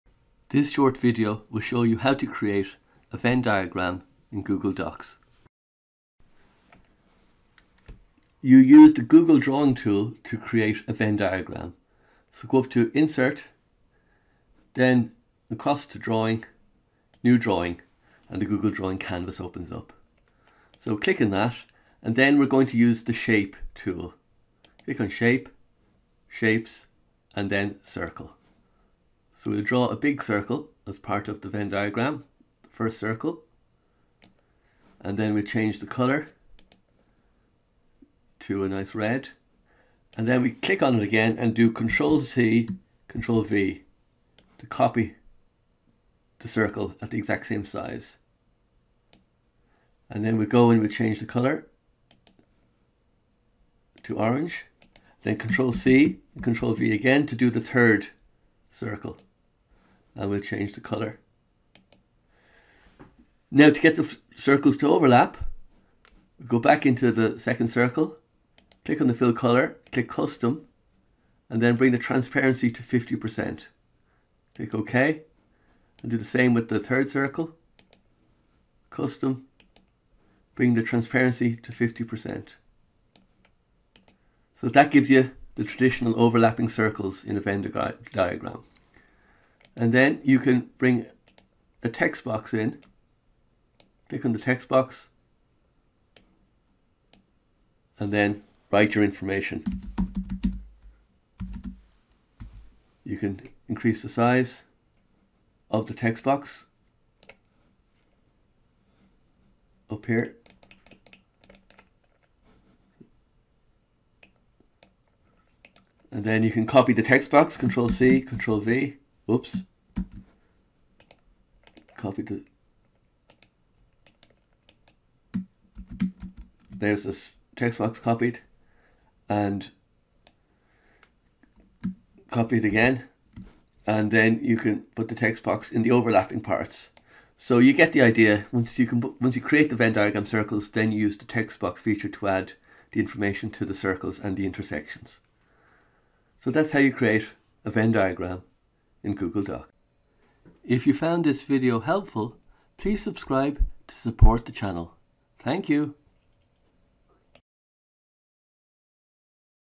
How to Create a Venn Diagram in Google Docs - Editable and Fillable
This video shows you how to make an editable Venn Diagram in Google Docs.
1. Open Google Docs.
2. Select 'Insert' and then 'Drawing' + 'New'.
3. User the shape tool to draw your first circle and change the colour.
4. Copy and paste the circle twice to make the two other circles and change colours to differentiate the circles.
5. Click on the second circle and change the colour background to semi-transparent by clicking the 'custom' button and to the same with the third circle.
6. Use the textbox tool to add fillable information to the circles and intersections.
7. Click 'Save and Close'.